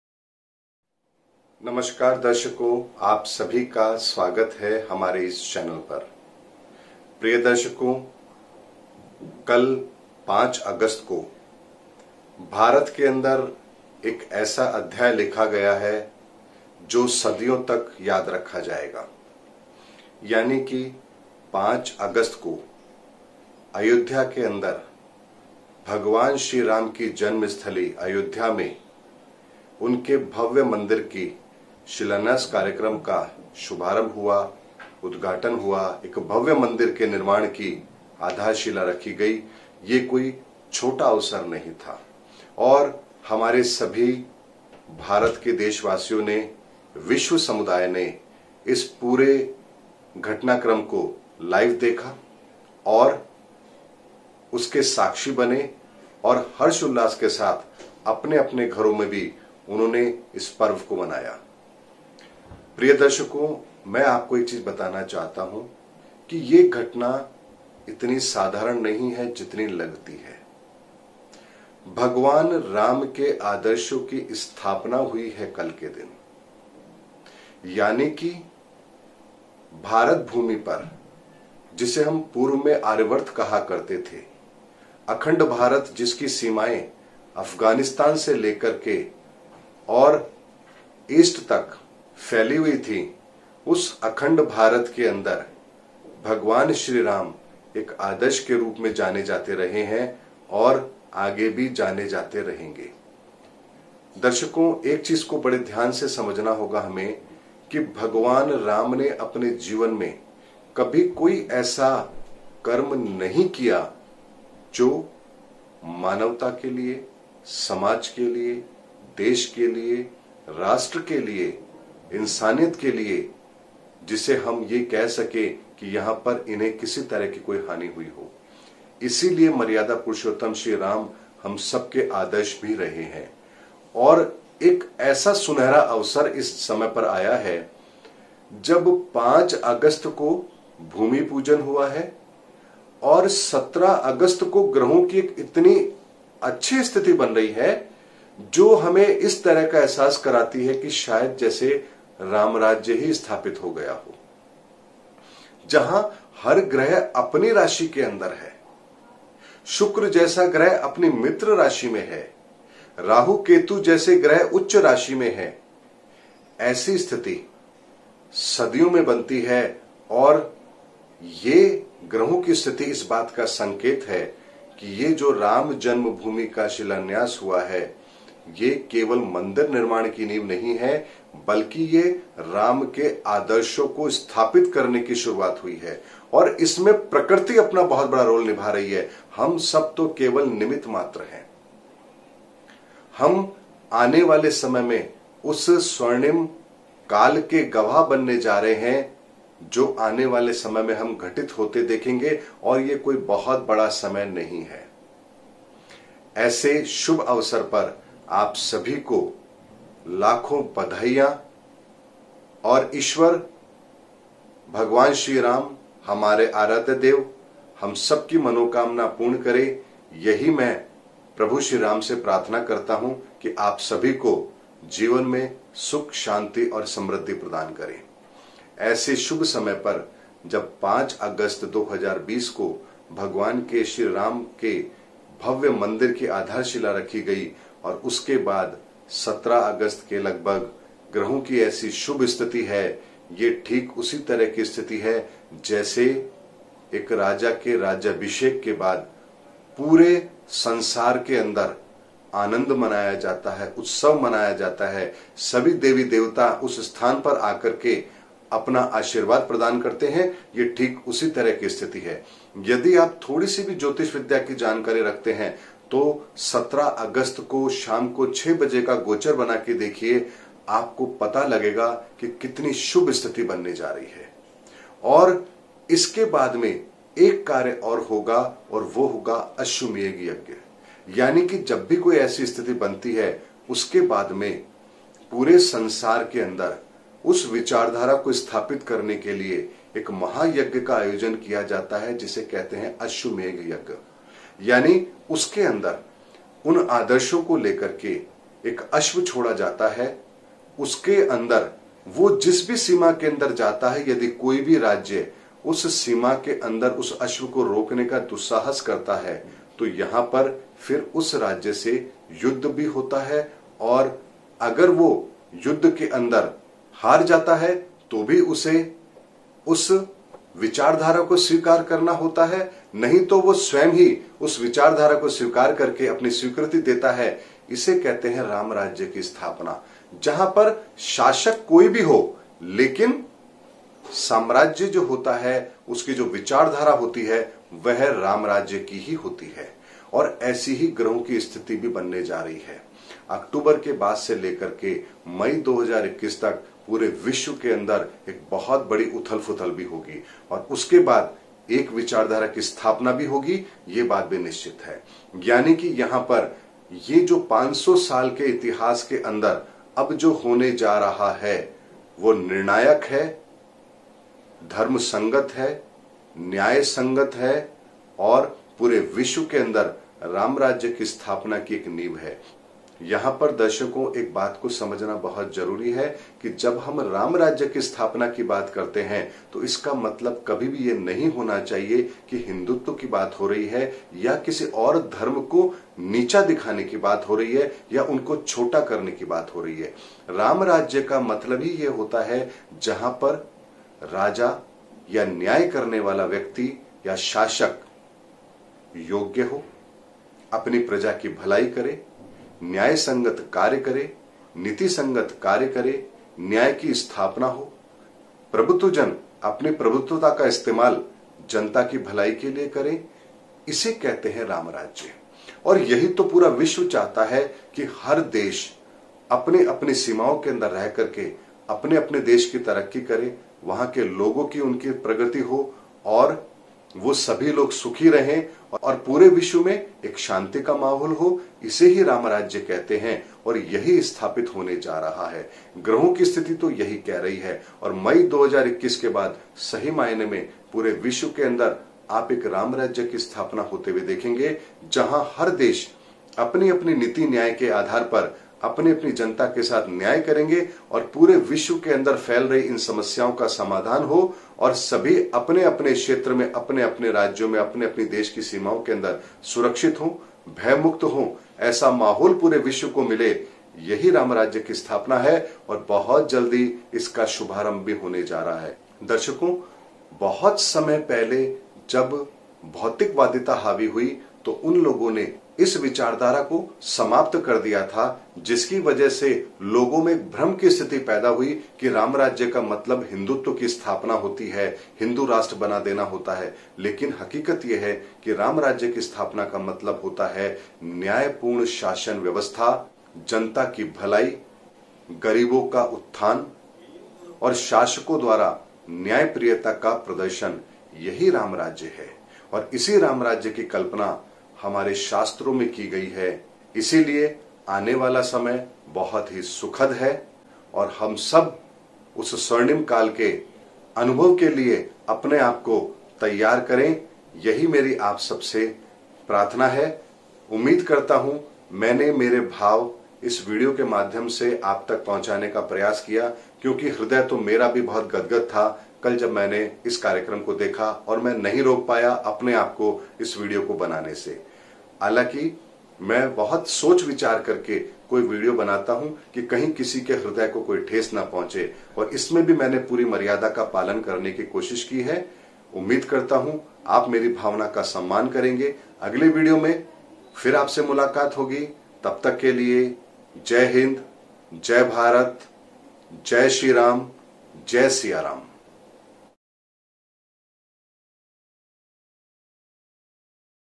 राम मंदिर का निर्माण एक नए युग की स्थापना
17 अगस्त 2020 होने जा रही हैं धर्म की स्थापना बहुत शुभ है ग्रहो की स्थिति

इस महामारी में सबसे बड़ा प्रभाव लोगों की जा रही है नौकरी हमें क्या करना है

अपनी कमियों को नहीं अपनी खूबी को देखने पर मिलते है जिंदगी में सफलता के रास्ते